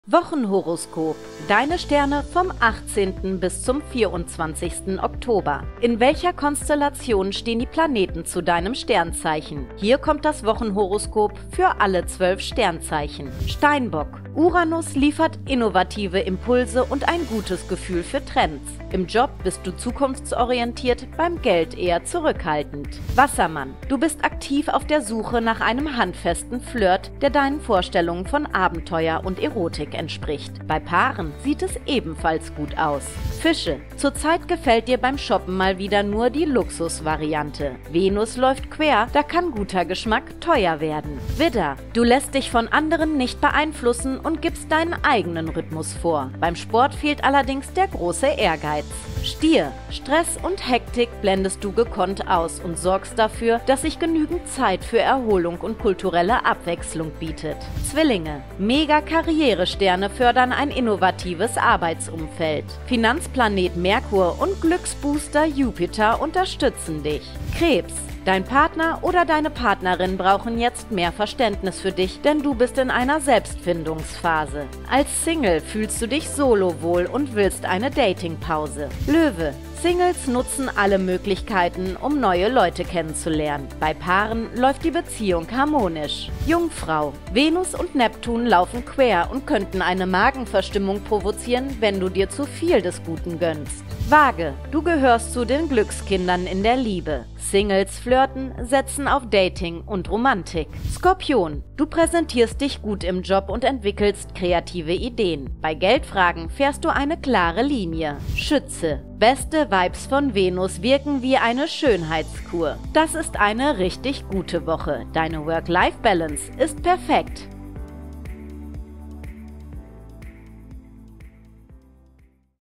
Wochenhoroskop: Deine Sterne vom 18. bis zum 24. Oktober
Wie sind deine Aussichten in Sachen Liebe, Job und Gesundheit in der neuen Woche? Wir verraten es dir im Wochenhoroskop.